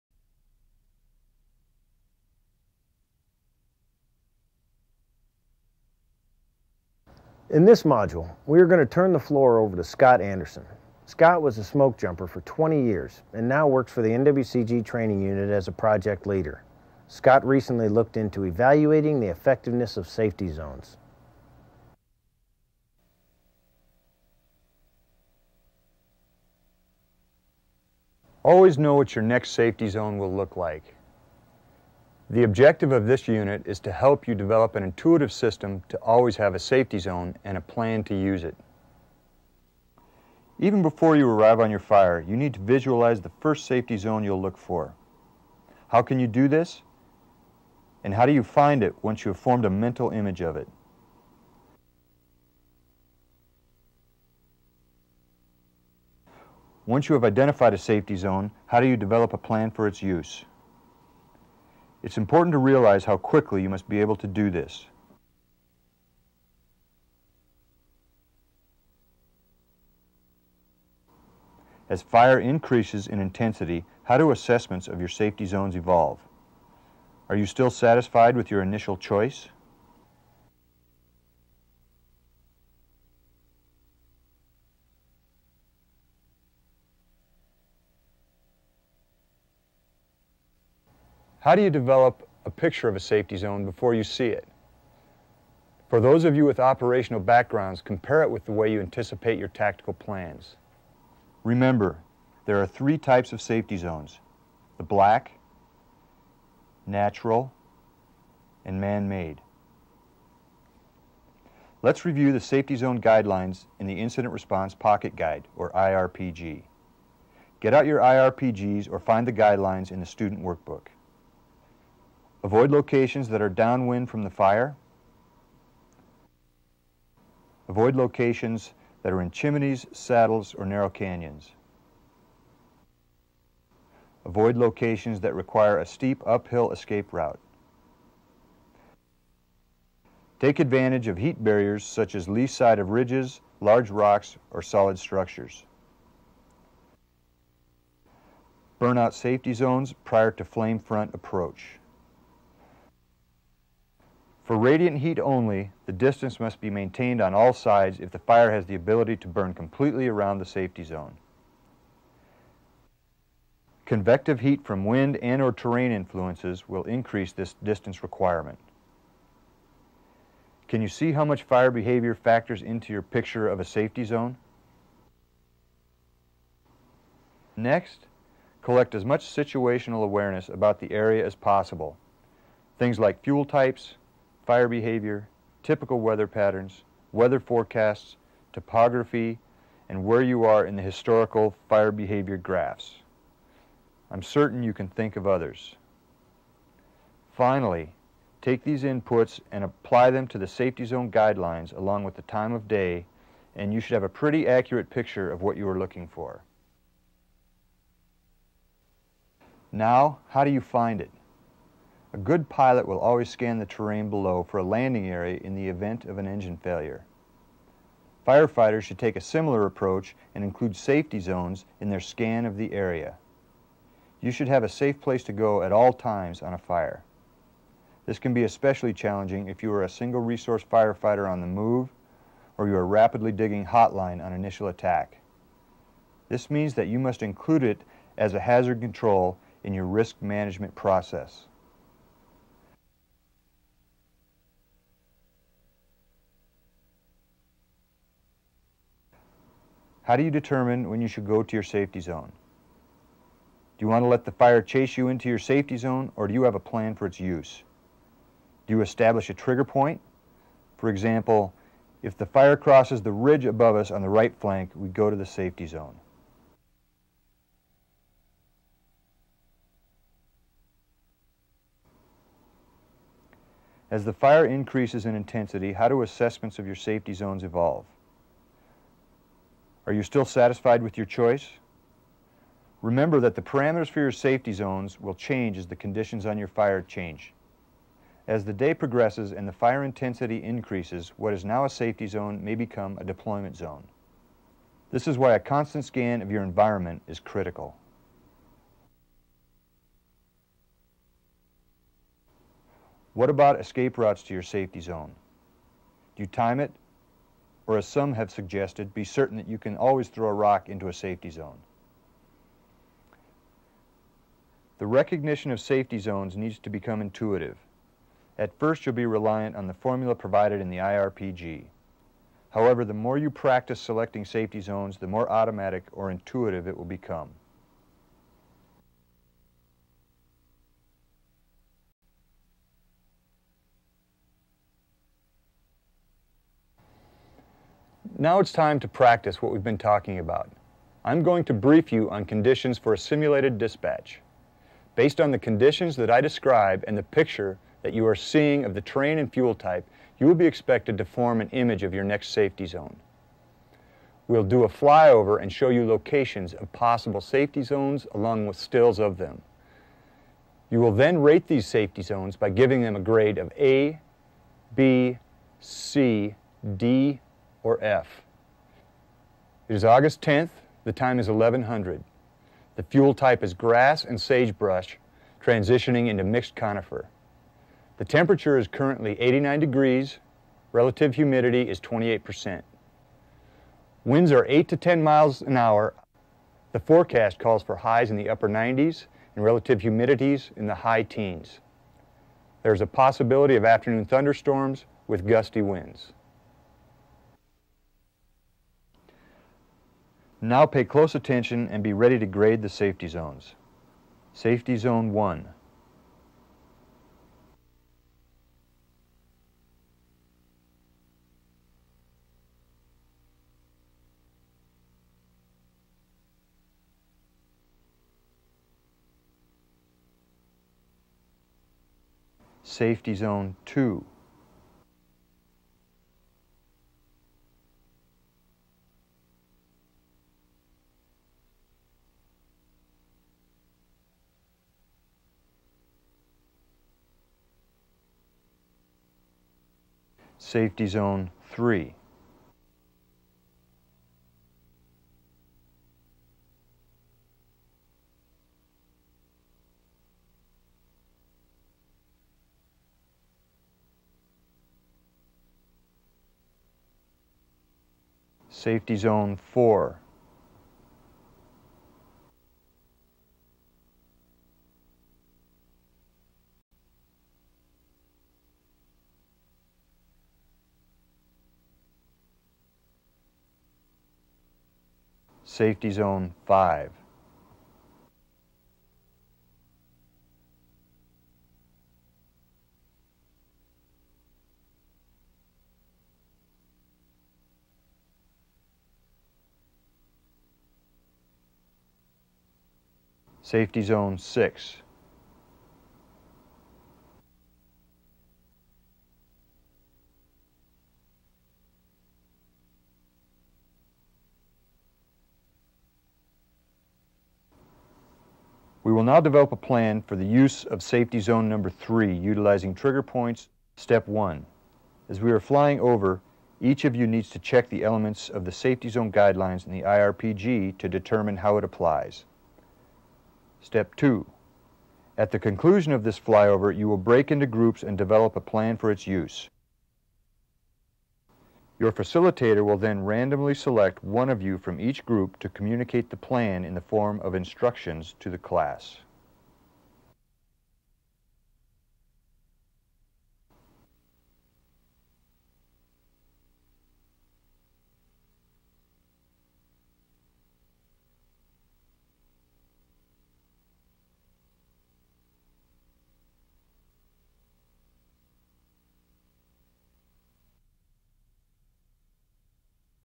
Situational Awareness - Part 18 - Safety Zones
Situational Awareness - Part 18 - Safety Zones - National Interagency Fire Center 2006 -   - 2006 Fireline Safety Refresher Training.